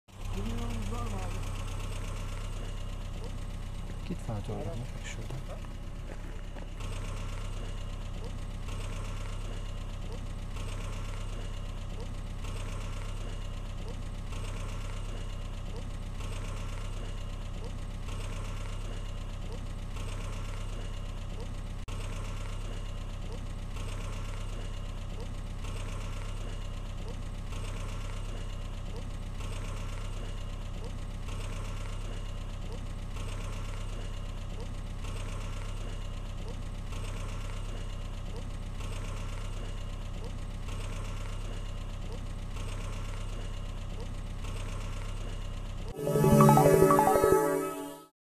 Çankırı’da nesli tükenmekte olan kırmızı Arap tavşanı görüntülendi
Çankırı'da Dünya Doğa ve Doğal Kaynakları Koruma Birliğinin (IUCN) "kırmızı liste"sinde yer alan nadir görülen Arap tavşanı doğaseverlerce görüntülendi.